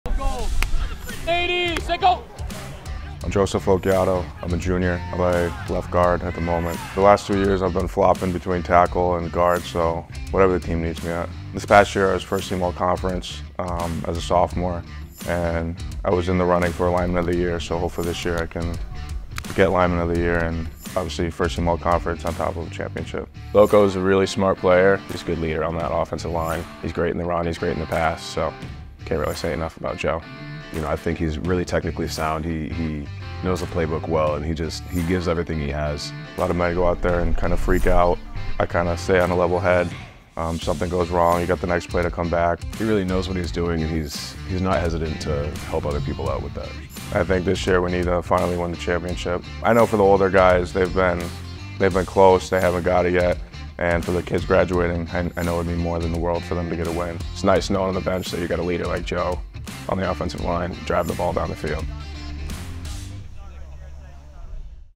Joe LoChiatto AKA "Loco"
The offensive linemen for Bentley was named first-team All-Conference and made the CSC Academic All-District team last season. Learn more about Joe Loco!